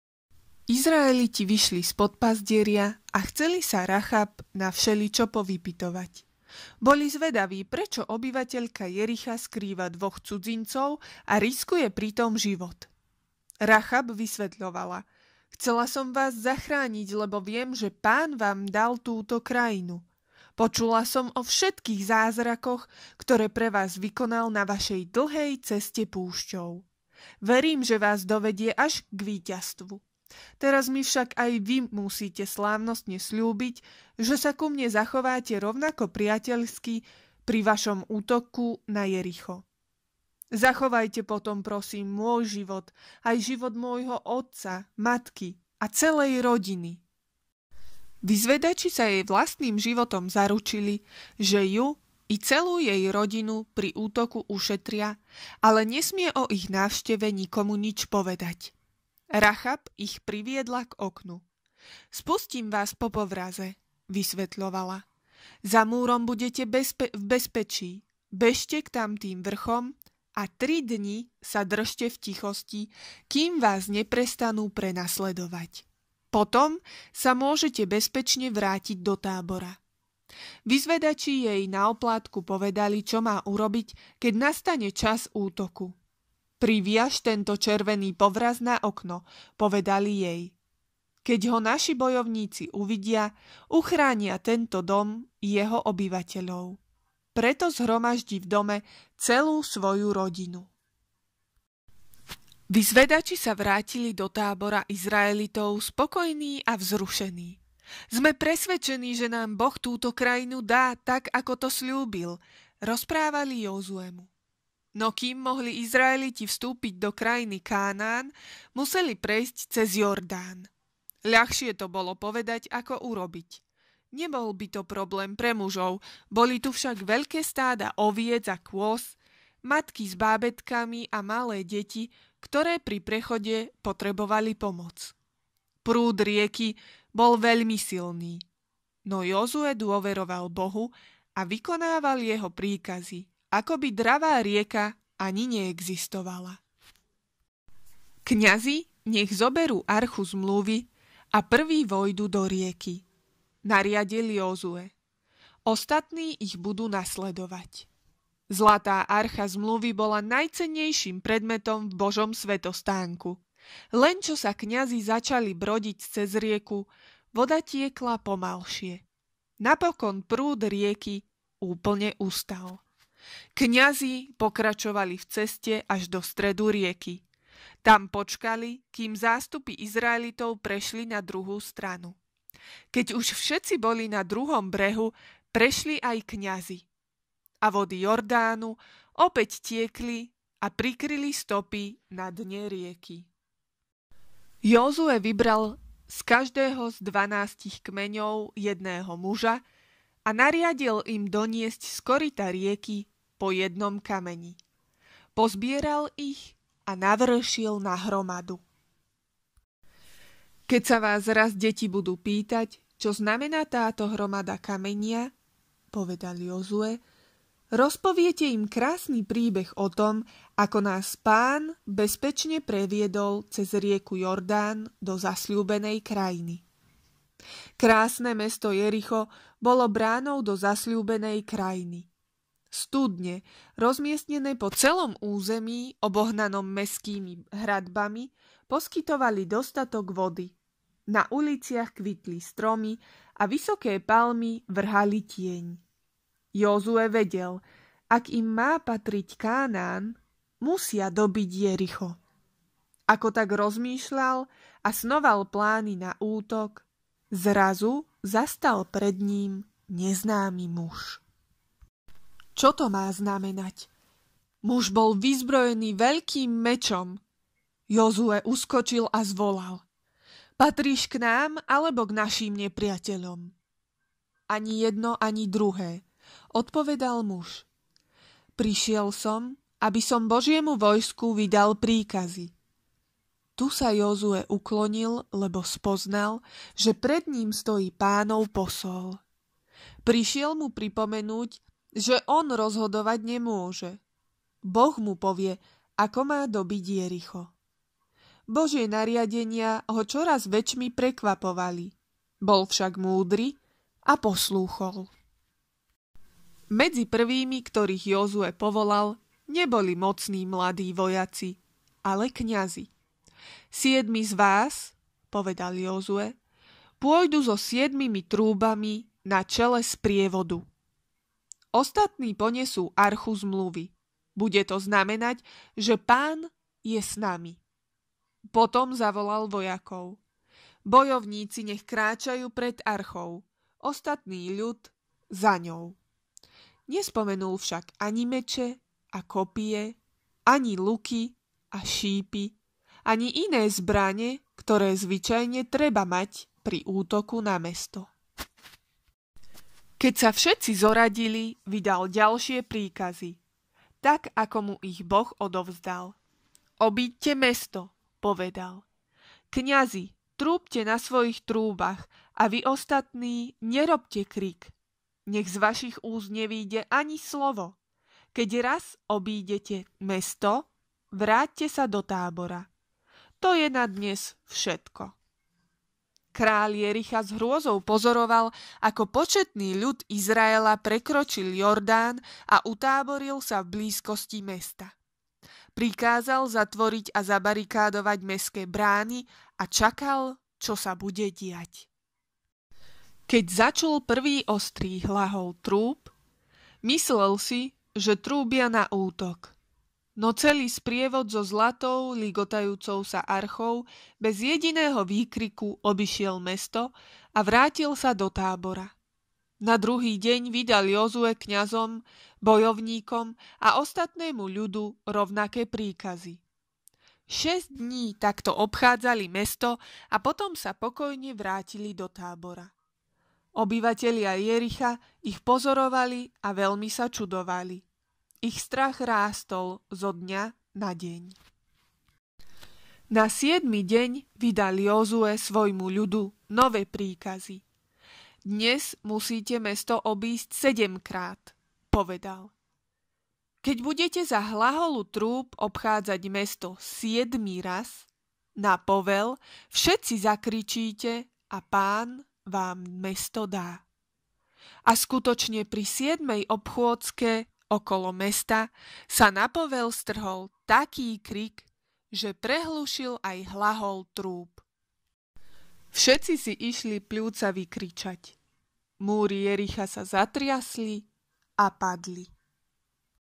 Múry Jericha
Ako Izraeliti porazia obyvateľov Jericha?